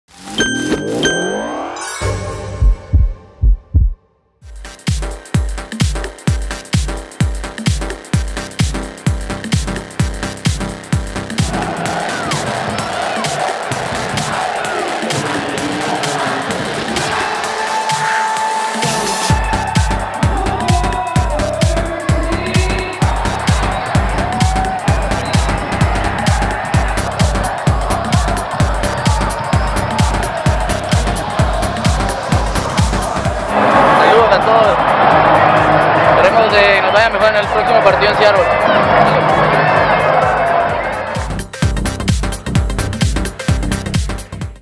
Marco Pappa y Seattle Sounders frente al Galaxy
Marco Pappa y el Sounders perdieron 1-0 en su visita al Galaxy en el Home Depot Center en la final de ida de la conferencia Oeste de la MLS.